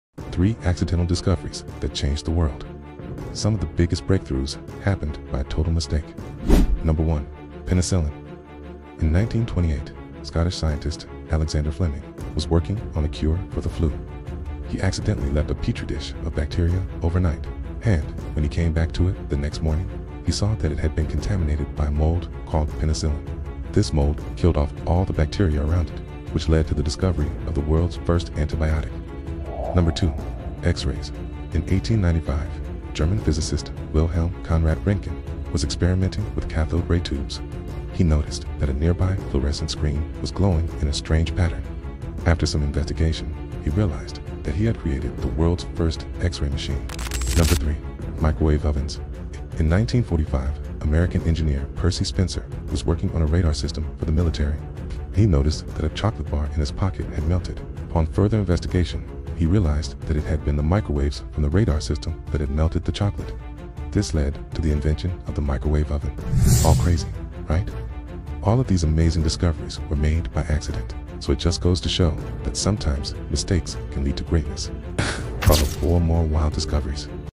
😲🔎3 Accidental Discoveries that changed the world🌎
Some of the world’s greatest discoveries weren’t planned—they were total accidents! 😲

Here are 3 mind-blowing inventions that happened by mistake. You’ll never look at your microwave the same way again!